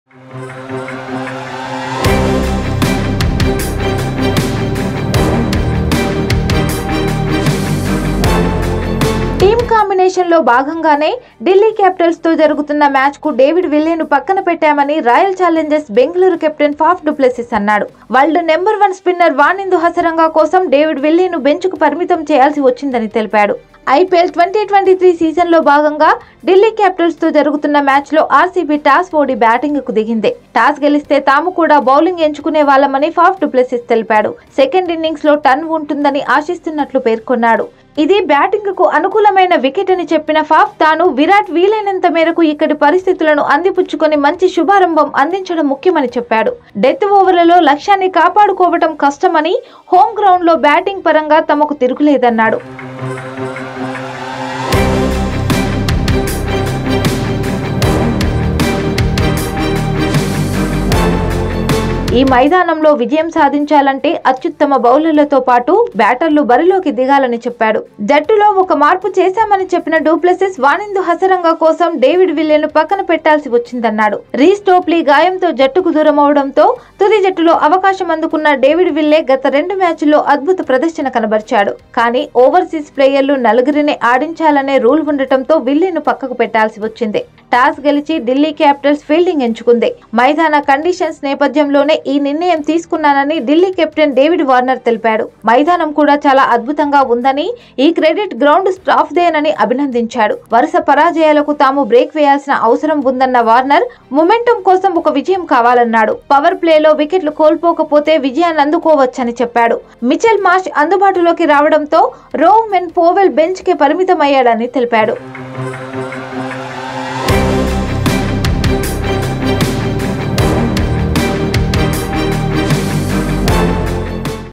IPL 2023 RCB vs DC ట్రోఫీ కోసమే కఠిన నిర్ణయాలు.. Faf Du Plesses | Telugu OneIndia
RCB vs DC Faf Du Plesses Reveals The Reason Why David Willey Not Playing Against Delhi.

టీమ్ కాంబినేషన్‌లో భాగంగానే ఢిల్లీ క్యాపిటల్స్‌తో జరుగుతున్న మ్యాచ్‌కు డేవిడ్ విల్లేను పక్కనపెట్టామని రాయల్ చాలెంజర్స్ బెంగళూరు కెప్టెన్ ఫాఫ్ డుప్లెసిస్ అన్నాడు.